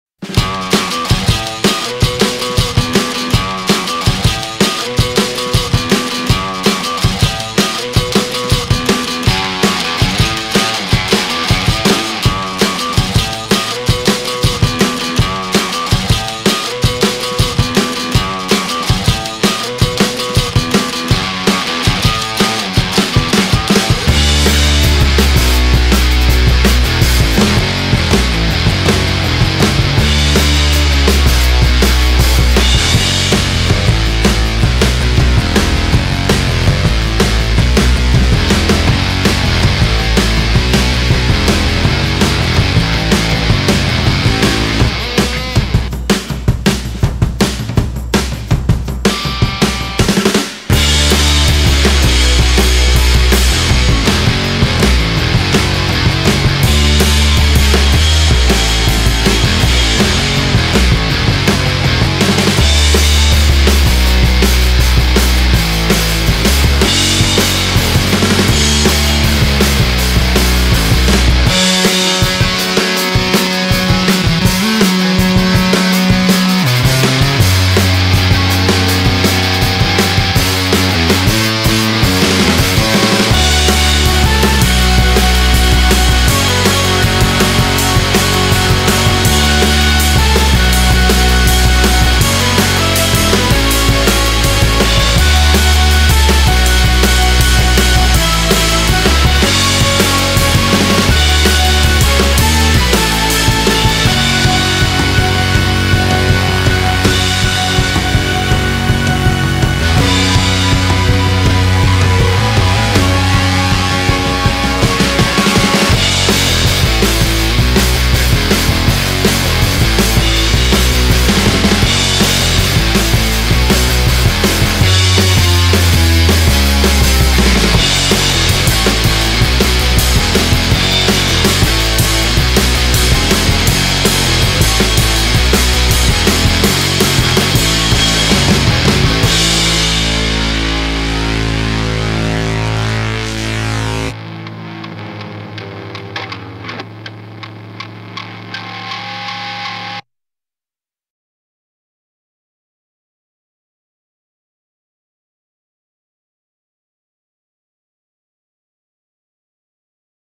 Motown Missile Rotisserie Time Lapse
This is another video for the restoration of the Motown Missile Cuda.  All pictures taken on location at Paul's Body Shop.  This video is mostly of us replacing the trunk floor , bottom quarter panels, and floor pan.